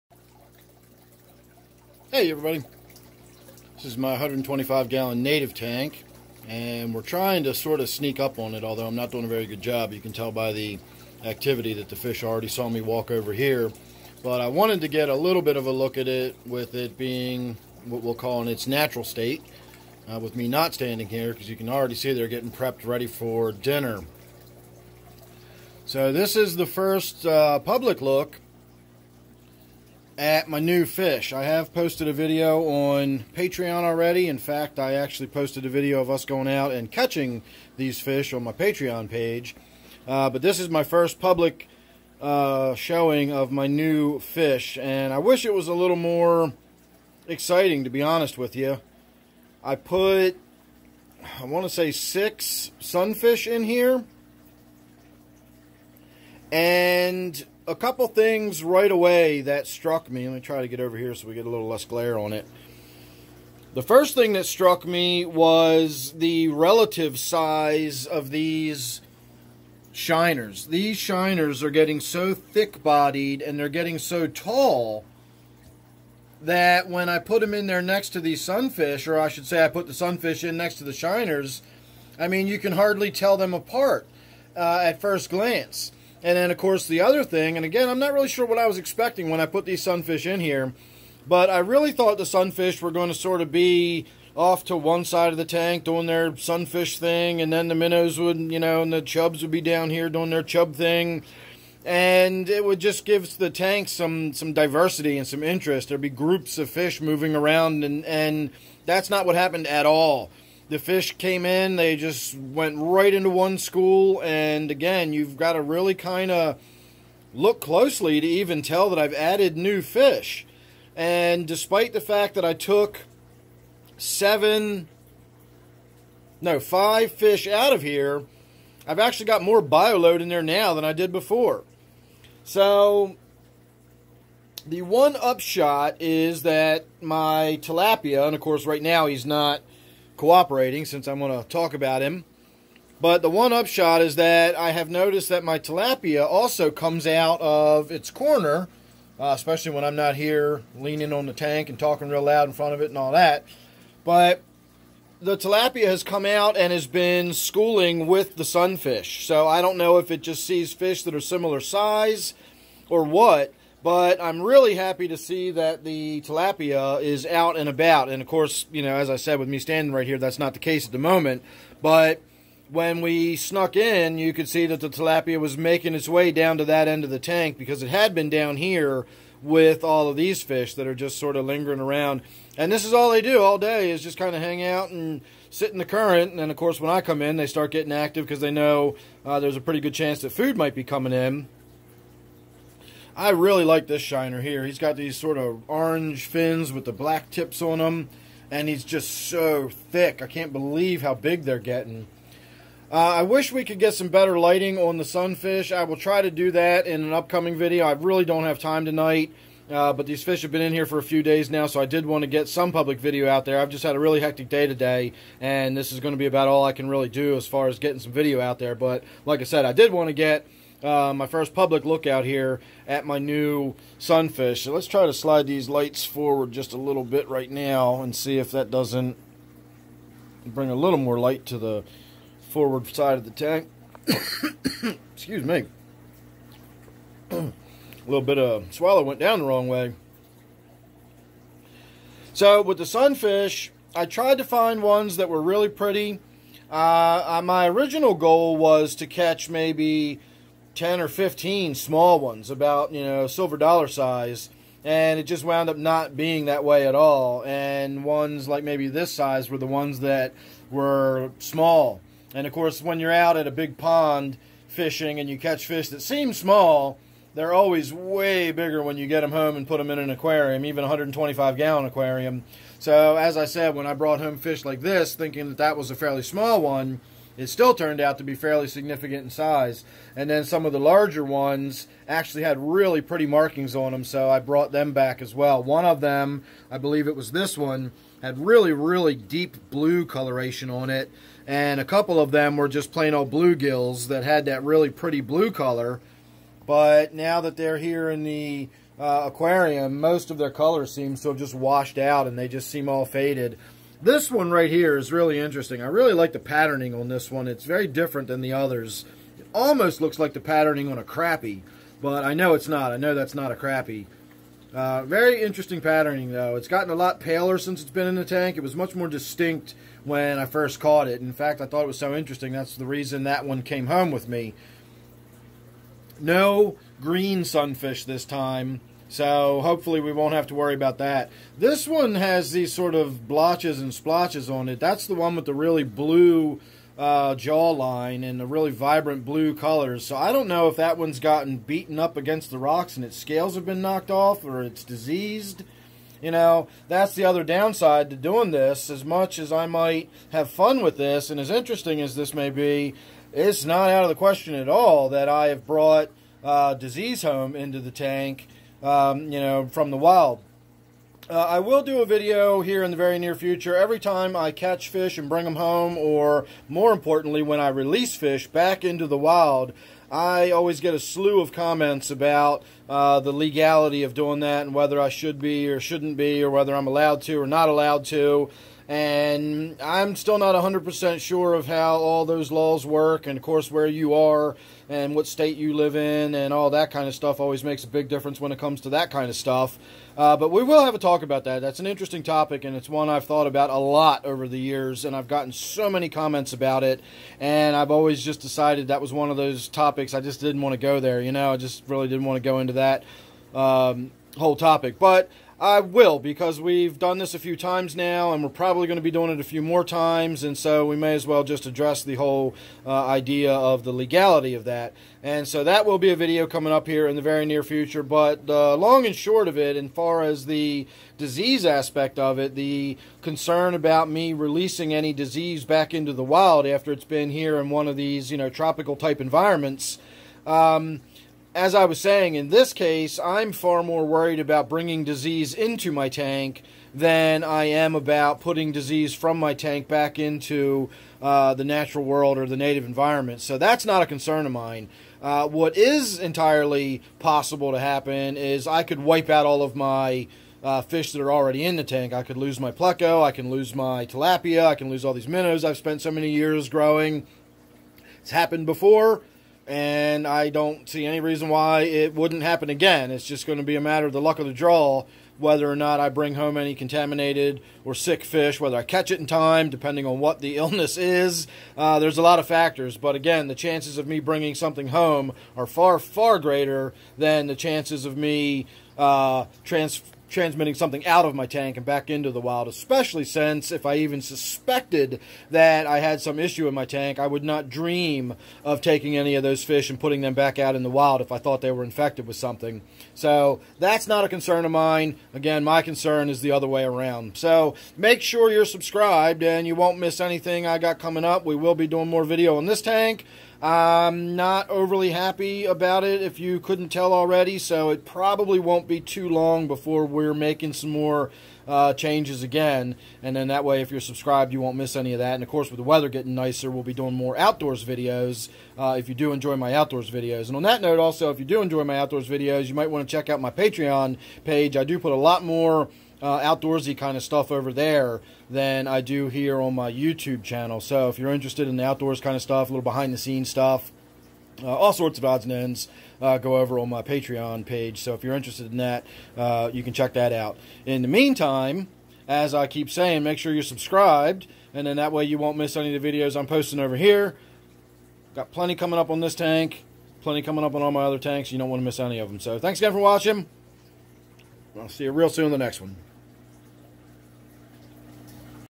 New Sunfish In My 125 Gallon Native Tank
I recently added several new Sunfish (bluegills) to my 125 gallon "Native Tank"